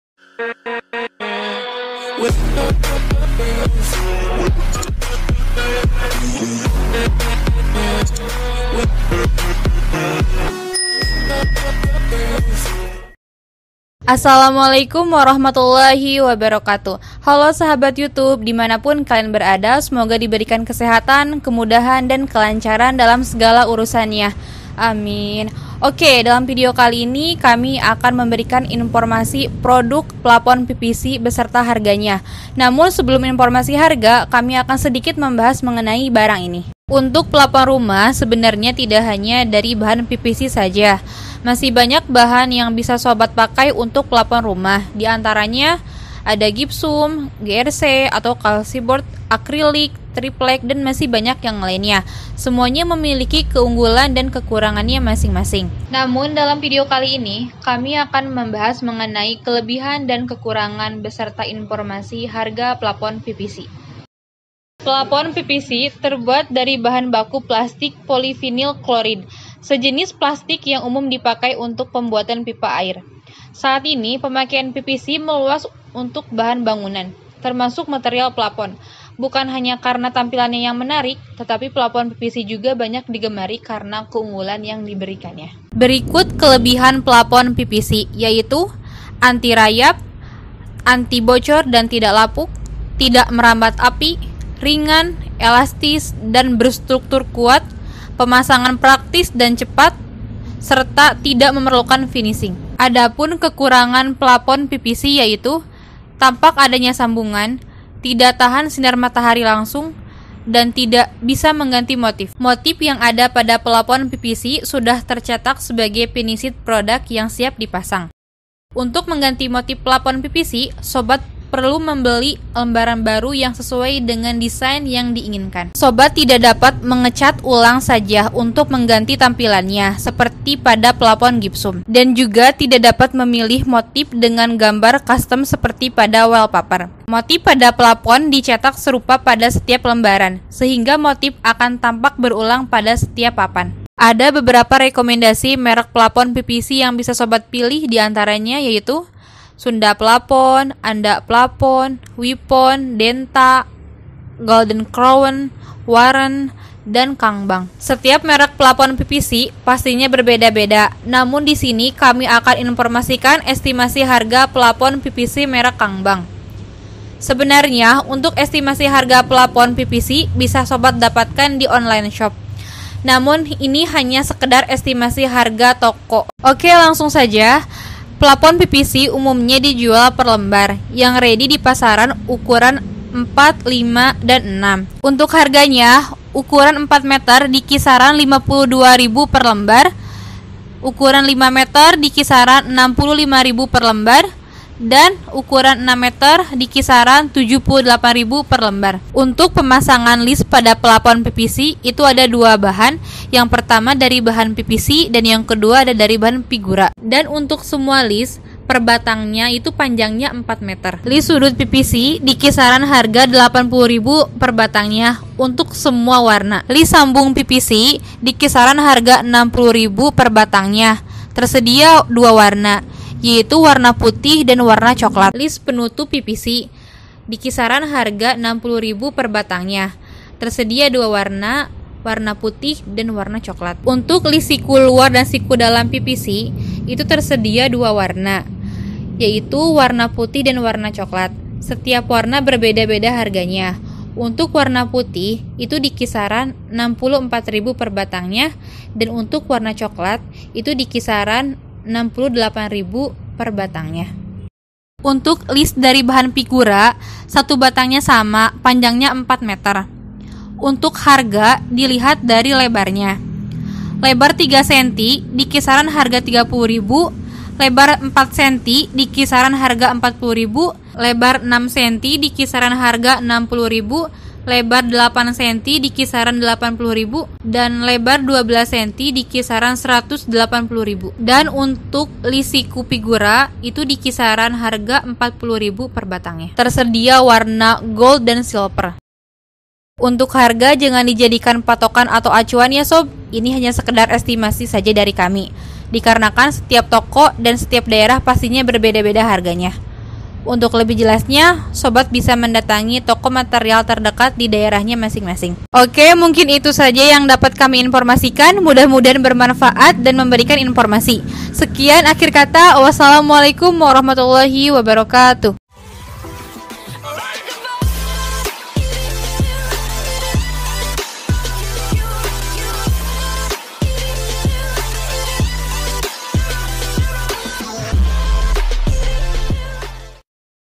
Rekomendasi merk plafon pvc‼️beserta info harga plafon pvc,lis pvc dan lis pigura
0:00 intro
0:36 sekilas plafon pvc
1:04 penjelasan plafon pvc
1:25 kelebihan plafon pvc
1:42 kekurangan plafon pvc
1:52 motif plafon pvc
2:24 rekomendasi merk plafon pvc
2:58 info harga plafon pvc
3:31 info harga lis pvc
4:14 info harga lis pigura
4:49 tambahan